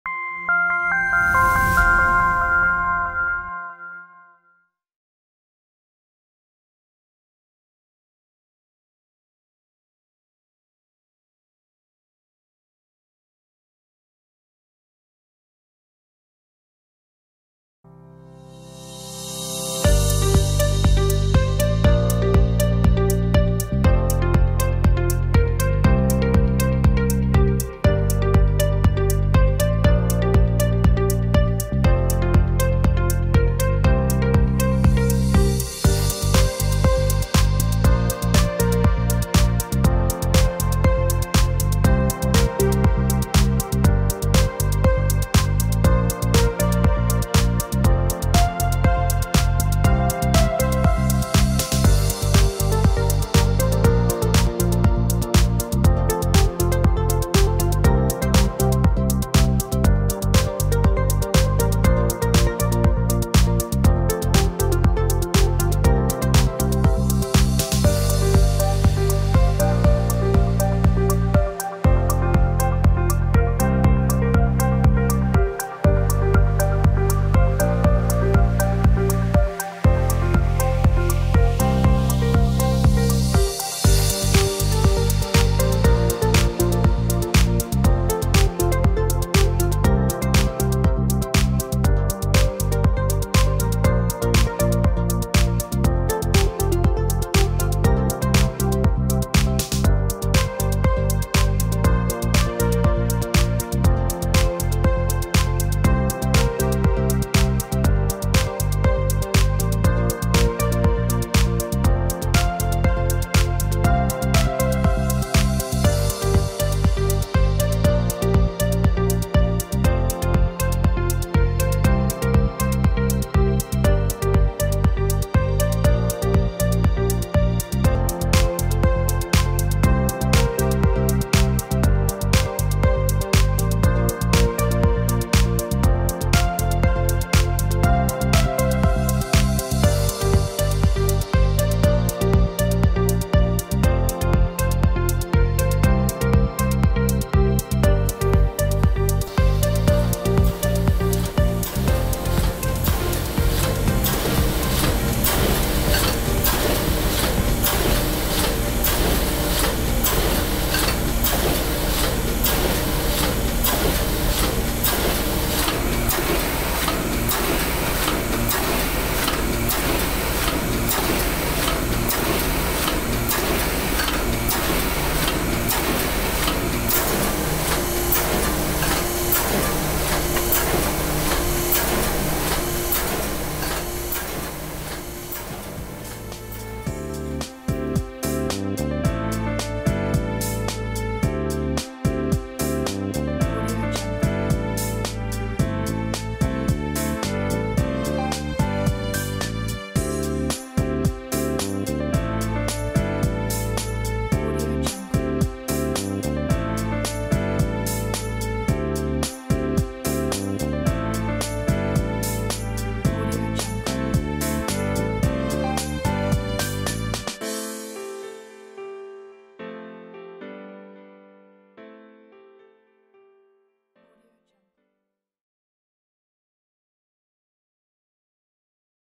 HSFF - Makeready Procedure - THERM-O-TYPE Corp.
Foil Stamping Equipment - NSF Excel Demonstration shows the procedure for selectively applying foil to sheets printed with toner. High Speed Foil Fusing (HSFF) does not require dies and can be used on most platen foil stamping presses.